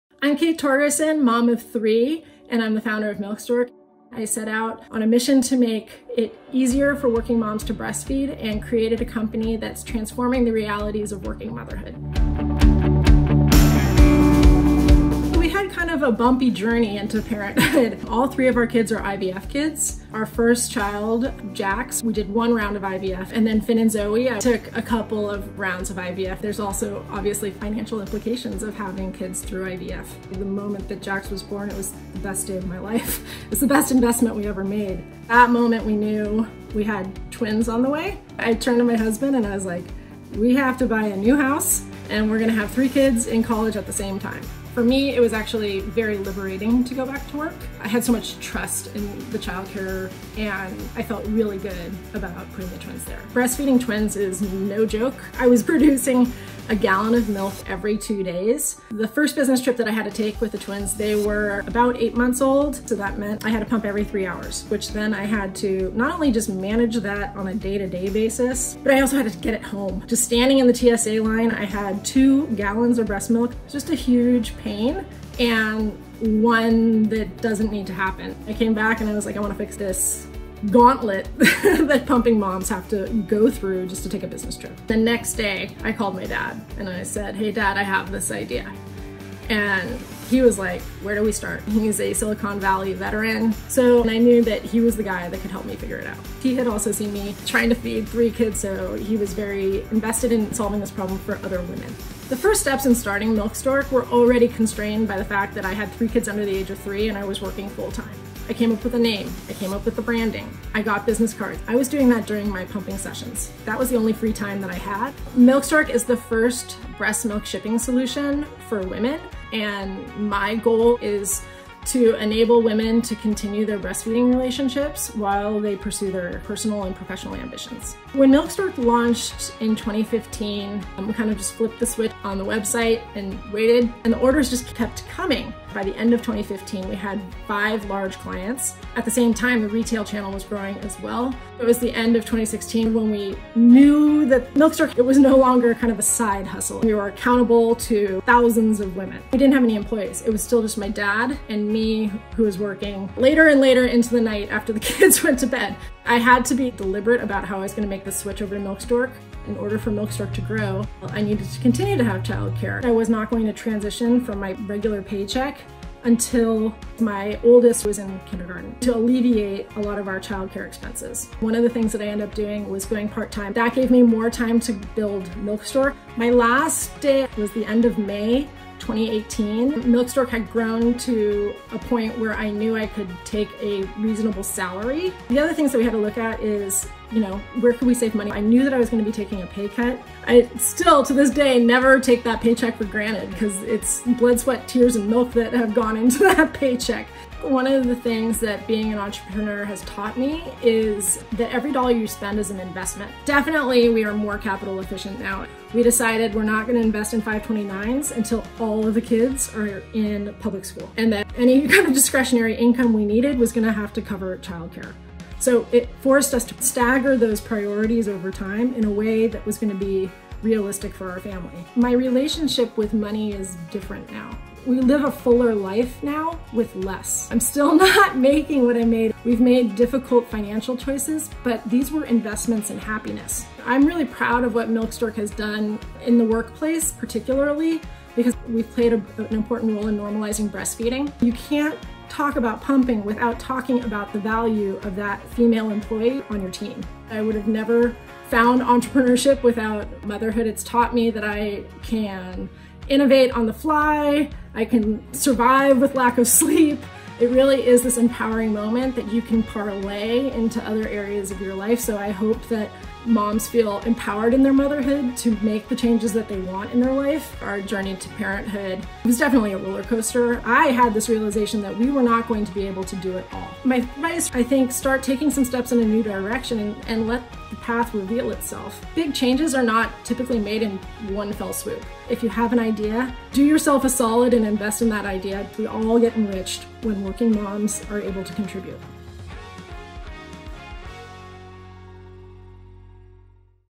Breastfeeding Moms Swear By The Company This Mom Built | Real Moms Real Money | Parents
Milk Stork founder, Kate Torgersen, was fed up with the woes of transporting breastmilk. After one fateful business trip, Torgersen was left to figure out a way to ship two gallons of breastmilk to her 8-month-old twins. Frustrated by the embarrassing and stressful process she experienced as she made her way through baggage screenings, Torgersen knew this was a problem she had to solve. Shortly after, Milk Stork was born—the first breastmilk shipping solution for women.

About Parents: We’re here to help moms and dads raise happy, healthy kids—and have A LOT of fun along the way.